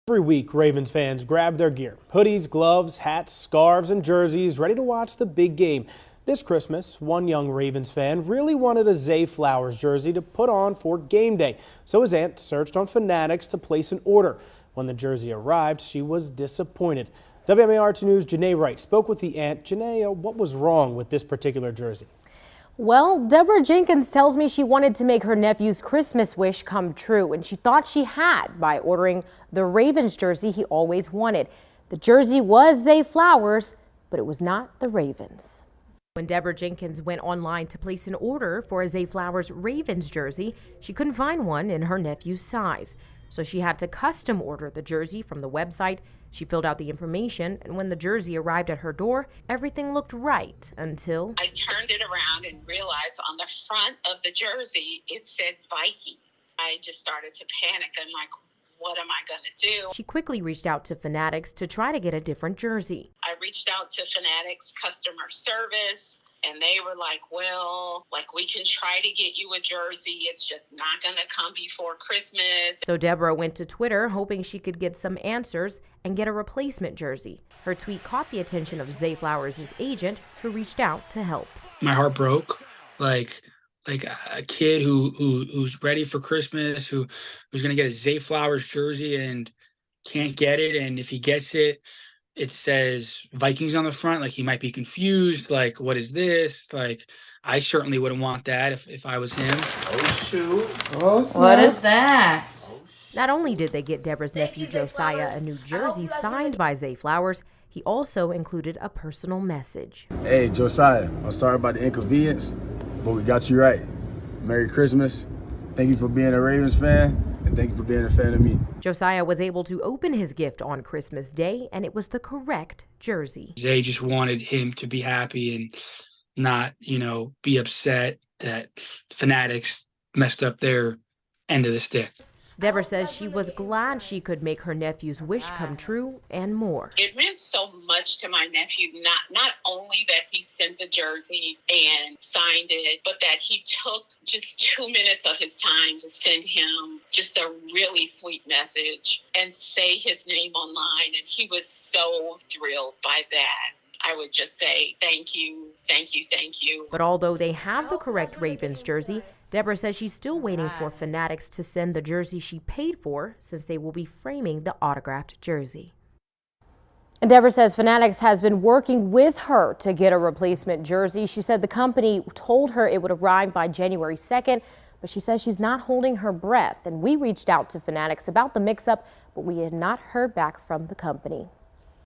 Ravens WR Zay Flowers surprises young fan who reportedly received wrong jersey
This holiday season, the flock got everything they could ask for after a huge win on Christmas night against the San Francisco 49ers, but one child got something a little more special.  Debra Jenkins got her nephew Josiah the one thing he wanted for Christmas, a Zay Flowers jersey. But when it came, it wasn't exactly what they had in mind.